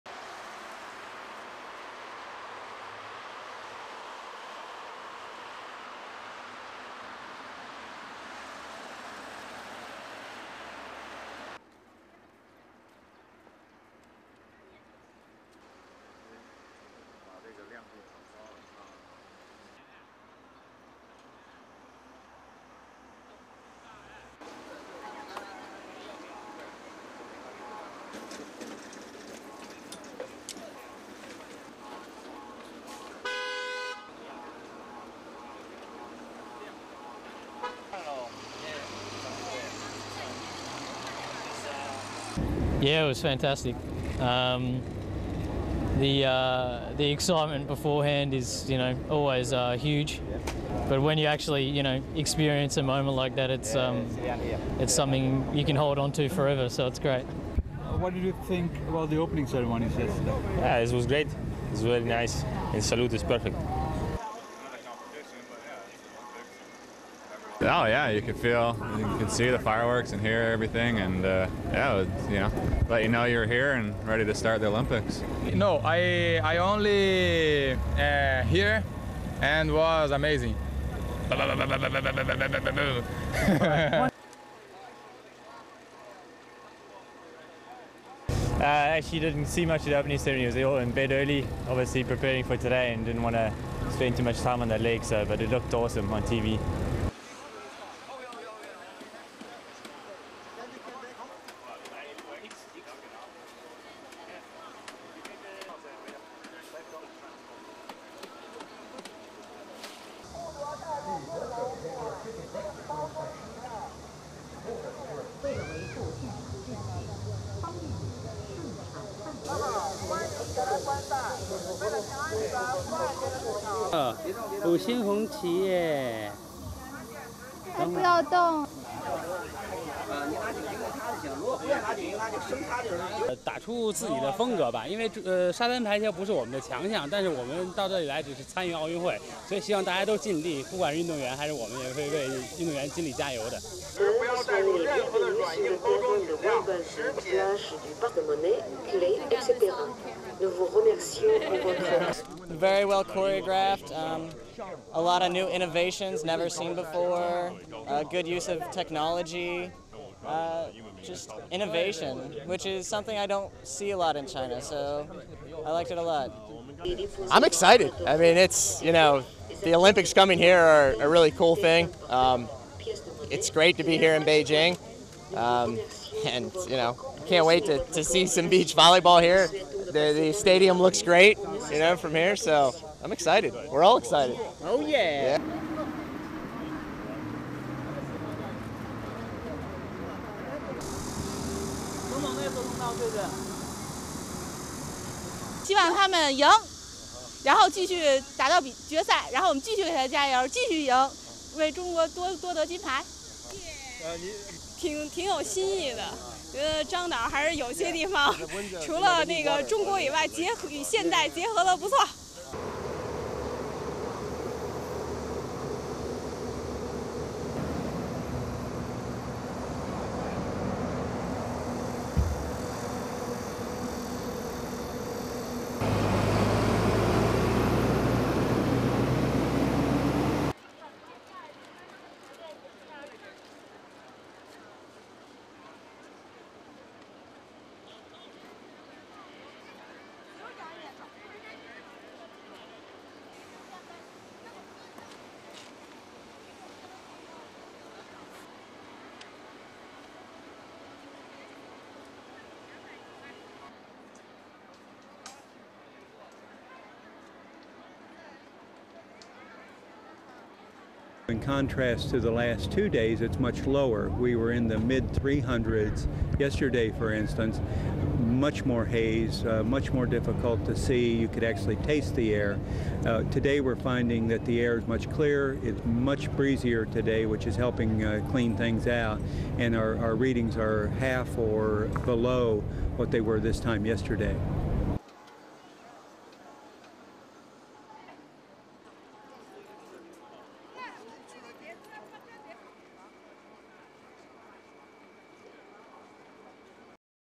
WRAP Day 1 of competition; beach volleyball queues; breeze helps cut haze
1. Pan from road to National Stadium, known as the Bird's Nest
2. Close up of torch
3. Bicycle on street
4. Wide of National Stadium
5. People taking pictures in front of National Stadium
7. Various of cycles being loaded on top of car
9. Set up of Australian cyclist Matt Lloyd
10. SOUNDBITE: (English) Matt Lloyd, Australian cyclist:
"It was fantastic. The excitement beforehand is always huge but when you actually experience a moment like that it's something you can hold onto forever so it is great."
11. SOUNDBITE: (English) Andiry Griyko, Ukrainian cyclist:
"It was great, it was very nice and the salute was perfect."
12. Set up shot of Hesjedal
13. SOUNDBITE: (English) Ryder Hesjedal, Canadian cyclist:
"You can see the fireworks and hear everything, lets you know you are here and ready to start the Olympics."
14. SOUNDBITE: (English) Luciano Mendonca, Brazilian cyclist:
"I only hear, and it was amazing."
15. Set up of South African cyclist David George.
16. SOUNDBITE: (English) David George, South African cyclist:
"I actually didn't see much of the opening ceremony as I was in bed early, obviously preparing for today and didn't want to spend too much time on the legs, but it looked awesome on TV."
17. Wide of Olympic village
18. Close up of guard at the gate
19. Close up of athletes walking out of the village
20. Wide of National Stadium
21. 'Beijing 2008' logo on stadium, policeman waiting outside, people near fountain
22. Low-angle shot of people walking
23. Close of hand painting child's arm
24. Mid of man holding child having arm painted
25. SOUNDBITE (Mandarin) Wang Ludi, Olympic Games spectator:
"I hope our players can play to their best level. China is not a powerhouse of beach volleyball but we like to come here to participate in the Olympics. I hope everyone involved will make their best effort, both the audience and the  athletes. We will make our best effort to cheer for the athletes."
26. Wide of crowds waiting at entrance to volleyball game
27. US supporters wearing Beijing 2008 Olympic flags, and US flags on their heads
28. SOUNDBITE (English) John Shaw, Olympic Games spectator from the United States:
(talking about the opening ceremony on Friday)
"Very well choreographed, a lot of new innovations never seen before, good use of technology -  just innovation which is something I don't see a lot in China, so I liked it a lot."
29. SOUNDBITE (English) Tim Curbin, spectator from the United States:
"I'm excited. I mean it's, you know, the Olympics coming here is a really cool thing.  It's great to be here in Beijing. I can't wait to see some beach volleyball here, the stadium looks great from here, so I'm excited. We're all excited."
30. Mid of Chaoyang Park security personnel checking passes at stadium entrance
31. Fans entering stadium
32. SOUNDBITE (Mandarin) Wu Xia, Olympic Games spectator:
"I hope our Chinese players can win today and I hope their success can continue on to the final, we will give full support to them. I hope China can win a lot of gold medals."
33. SOUNDBITE (Mandarin) Tian Zhenzhen, Olympic Games spectator:
"The opening ceremony is full of innovations. It shows that director Zhang Yimou is indeed a great director. He has done a great job to combine the various cultures and colours, ancient and modern, oriental and western."
34. Various of athletes' bus arriving
35. Sculpture outside National Stadium, pan to stadium
36. Olympic flame burning in cauldron atop stadium
37. Associated Press journalist Brian Horton walking on Olympic Green
38. Various of Horton holding air testing equipment
STORYLINE: